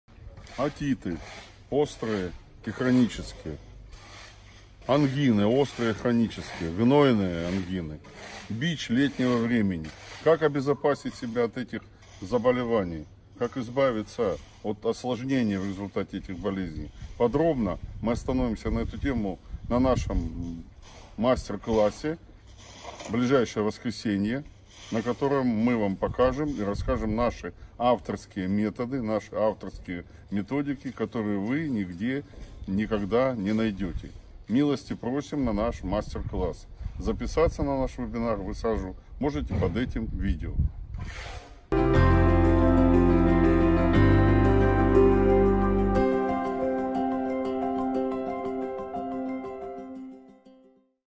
Отиты, ангины острые и хронические. Как обезопасить себя и защититься от последствий.
✔️ Регистрируйтесь на вебинар Эфир будет в онлайн, поэтому вы можете смотреть из любого города и страны, где бы вы не находились.